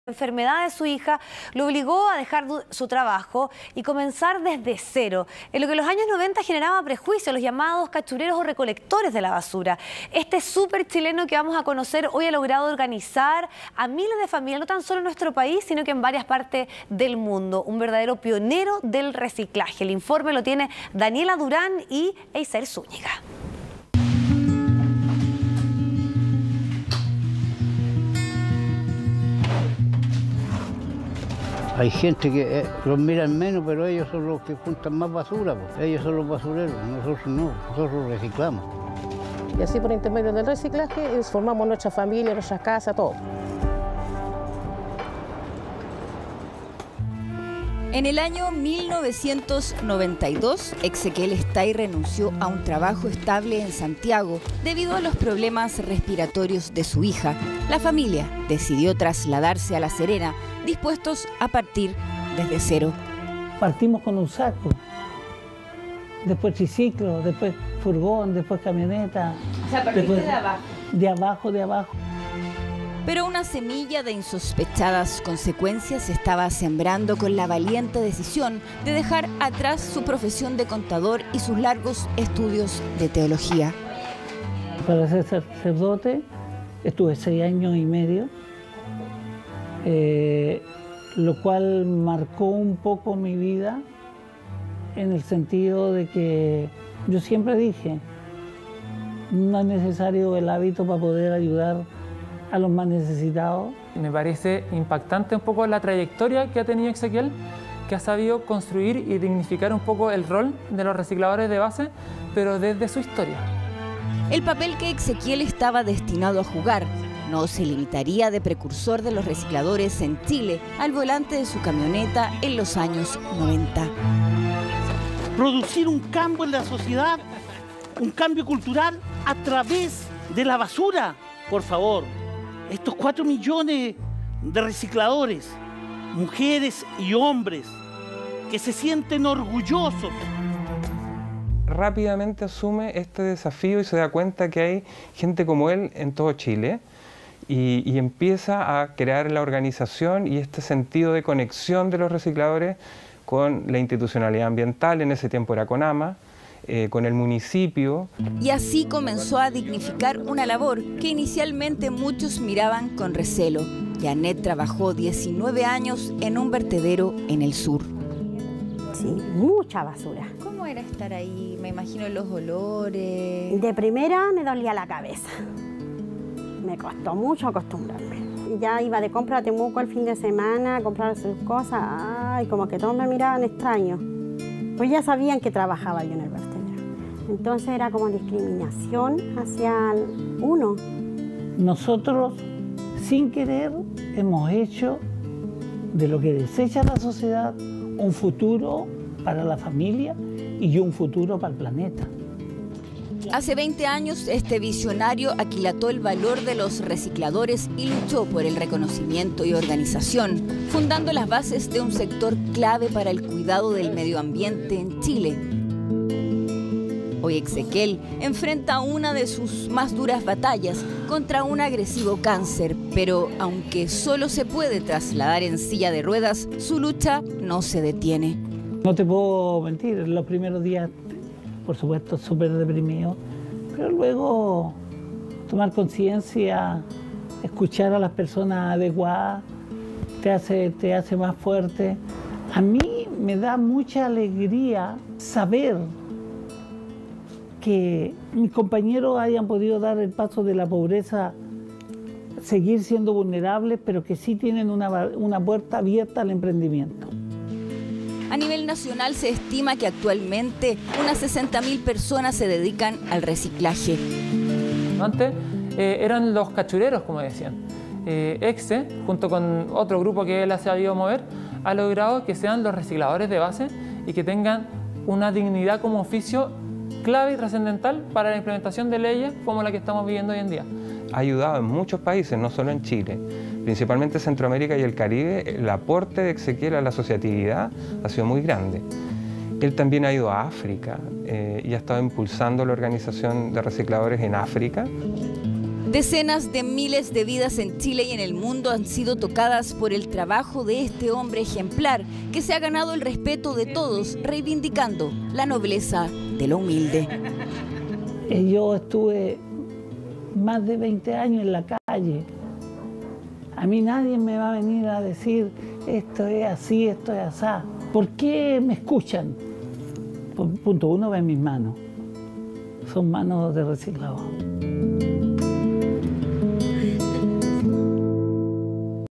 SUPERCHILENOS | Exequiel Estay, el pionero del reciclaje que organizó y dignificó el rubro en Chile
Desde hace más de 20 años, Exequiel ha luchado incansablemente por los derechos y el reconocimiento de los recicladores base. Este año, recibió un reconocimiento al ganar el primer premio Desafío Tierra gracias a su aporte al cuidado del medio ambiente.